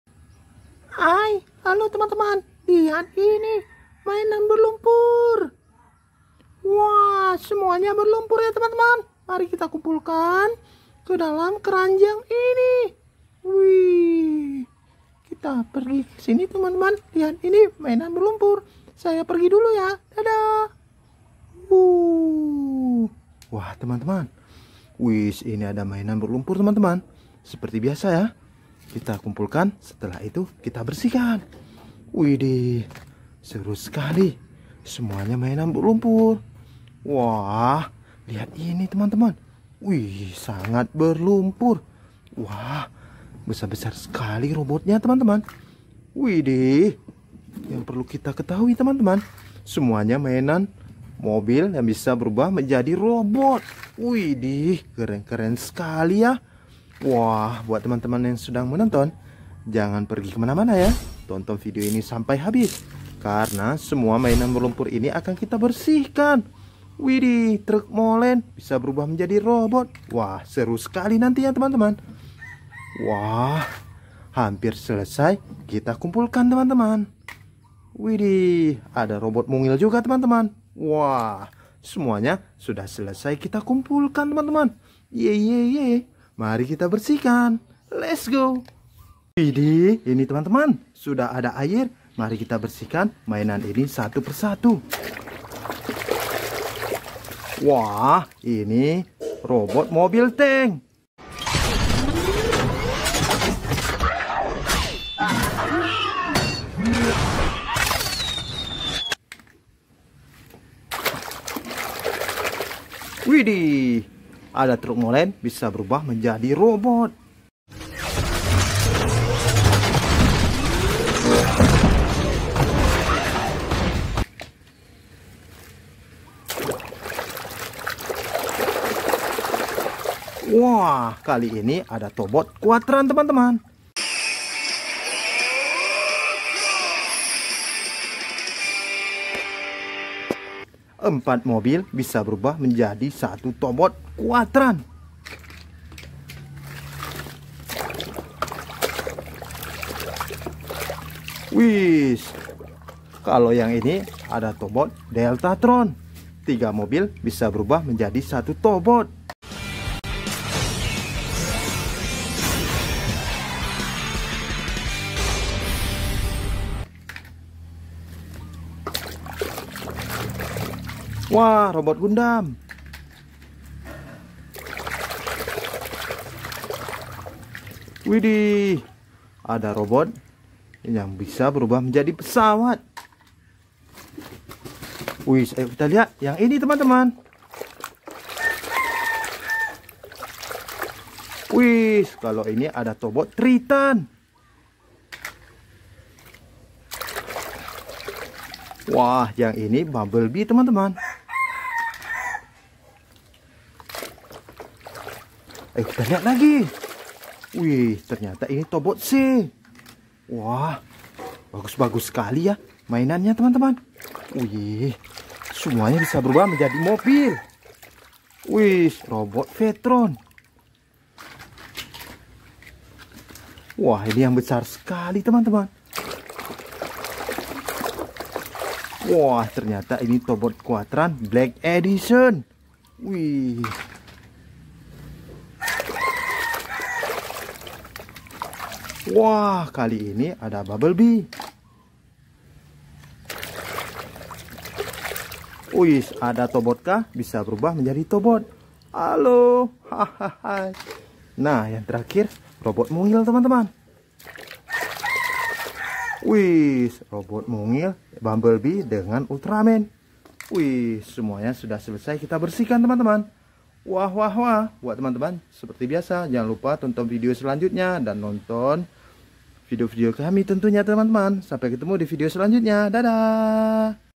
Membersihkan Mainan Robot & Tobot, Mobil Robot, Robocar, Gundam, Mainan Mobil.
Di videokali ini kita akan Membersihkan Mainan Robot & Tobot, Mobil Robot, Robocar, Gundam, Mainan Mobil.